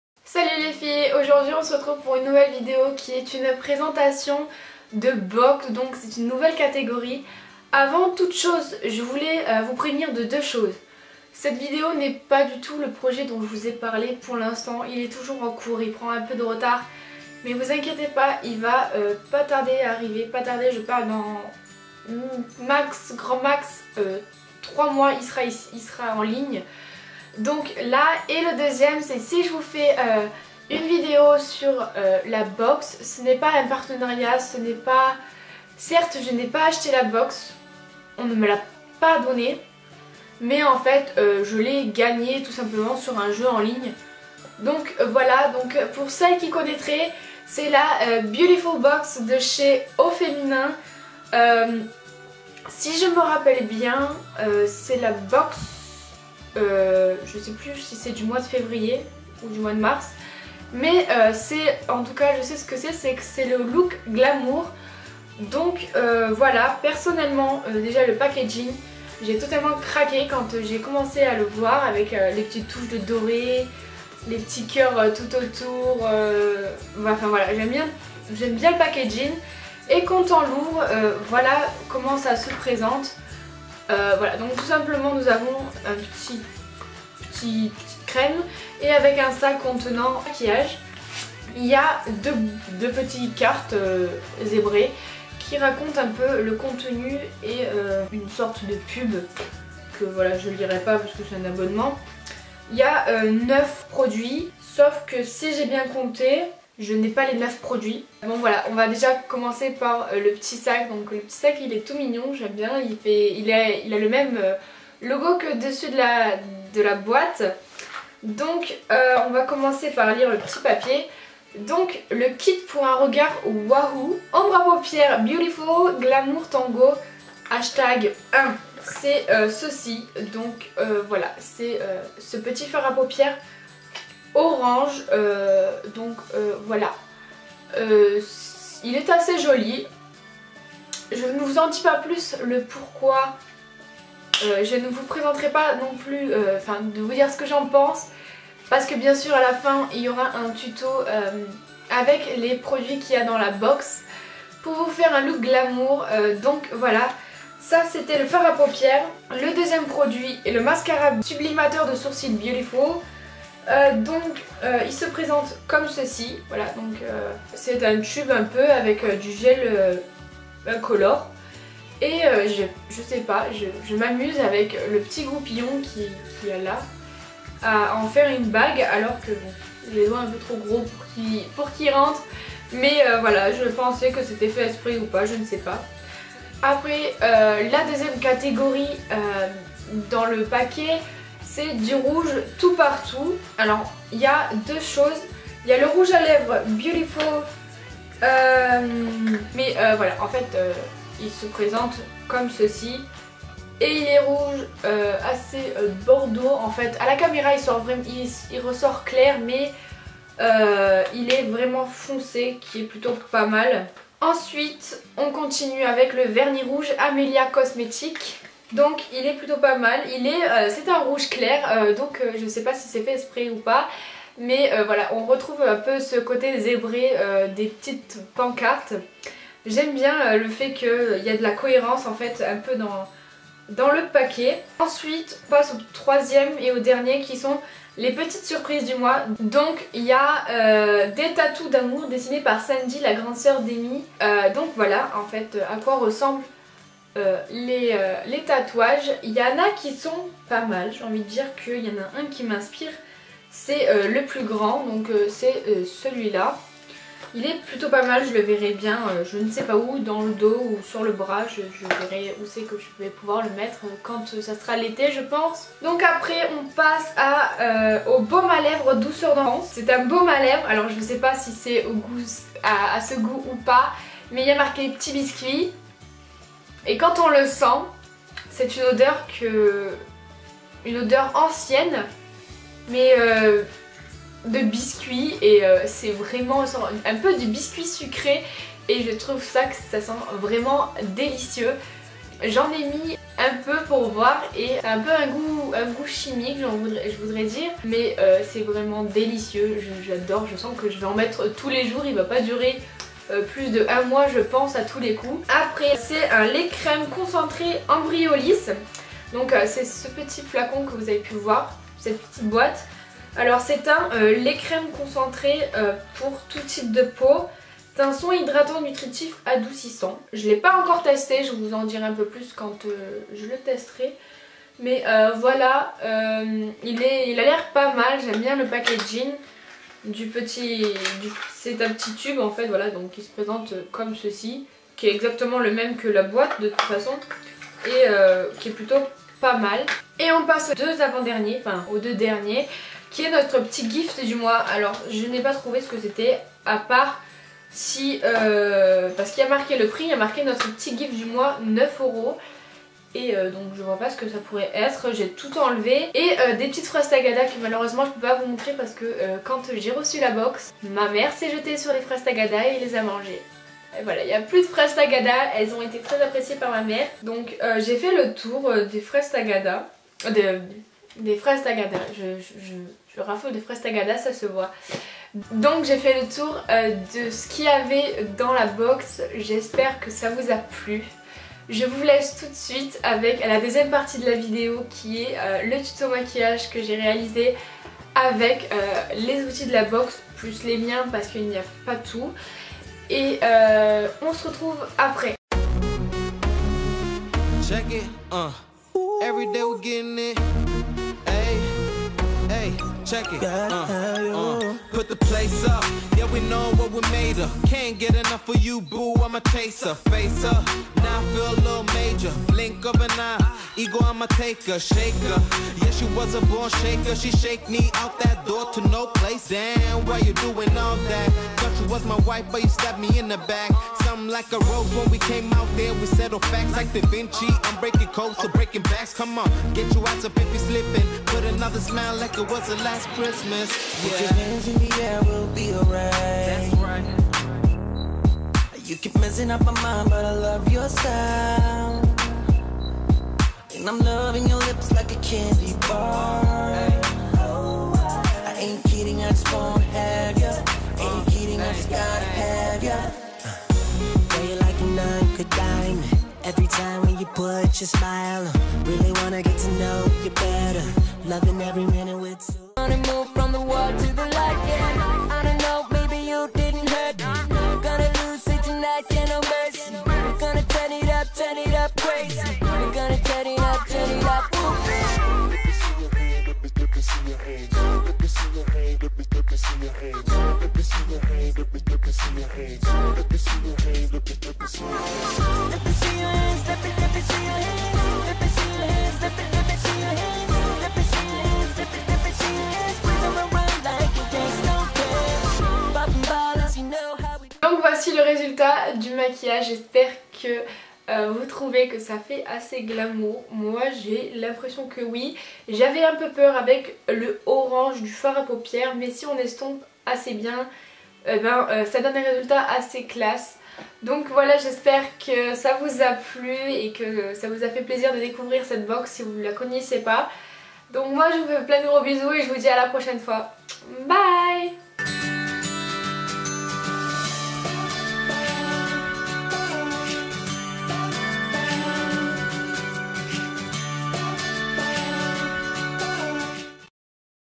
[ Présentation de Box n°1 ] Beautiful Box de Janvier 2016
Hello les filles aujourd'hui on se retrouve pour un unboxing de la box de aufeminin

N'hésiter pas a me poser des questions et me dire quel type de vidéo voulez vous voir sur ma chaîne.

ღ pour me suivre :

ღ Snapchat: ღ

margaux.france

ღ Facebook ღ

ღInstagram :ღ

ღTwitter:ღ